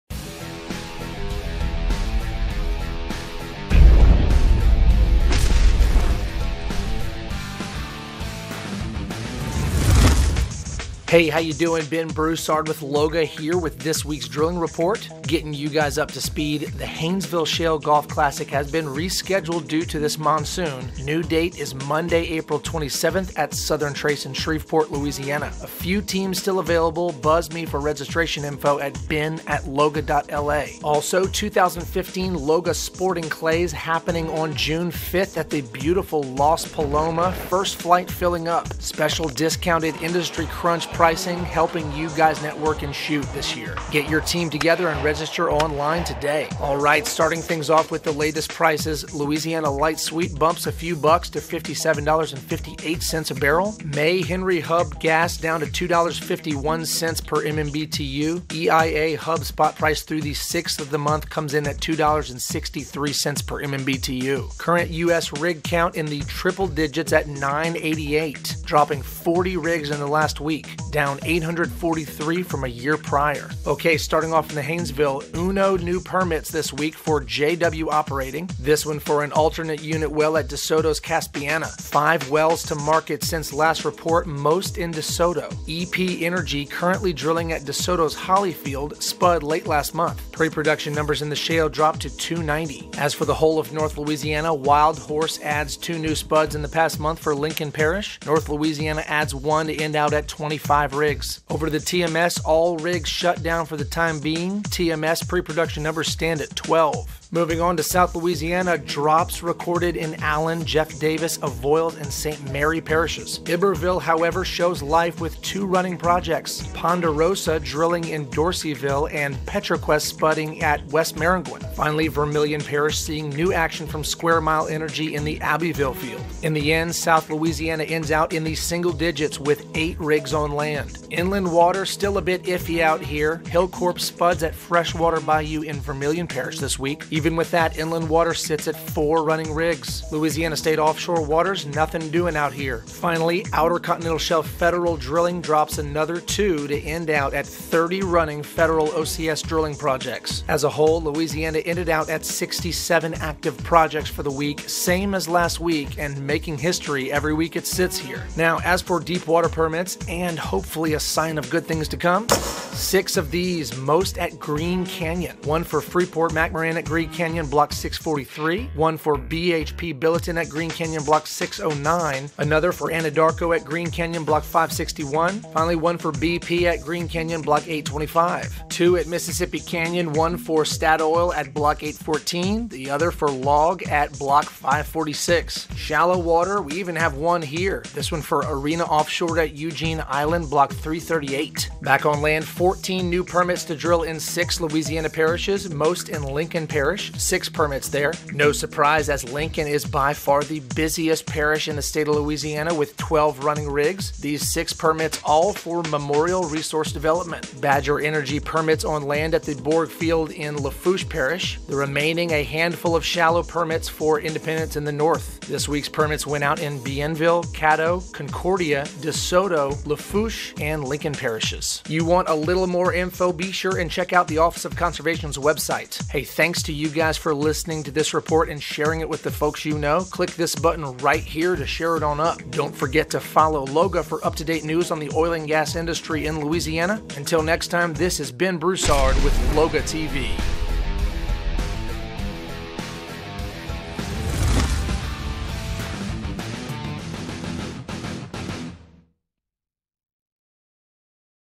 LOGA Drilling Report for Louisiana - 4.13.15
LOGA's Marketing Director Ben Broussard brings you the LOGA Drilling Report. He will update the current drilling activity in the Haynesville Shale, North Louisiana, Tuscaloosa Marine Shale, South Louisiana Land, Inland Waters and the Gulf Of Mexico.